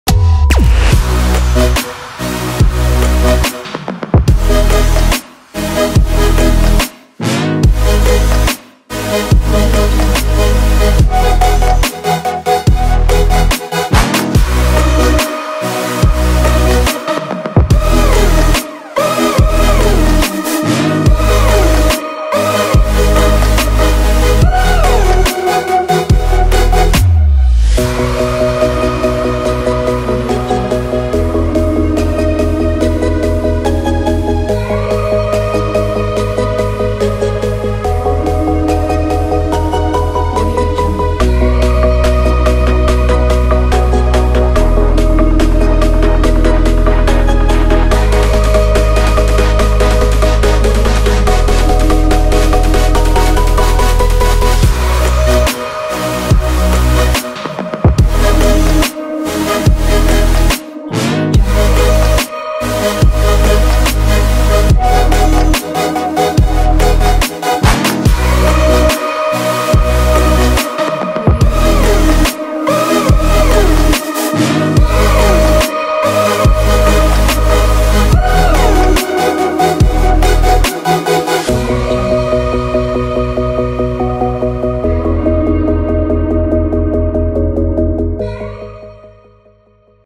Modern Text Animation - Titles | After Effect Template
Modern Text Animation
8 scenes
+8 RGB version scenes
After Effects CC2022
4K Resolution (3840×2160)
Fast render
Friendly Interface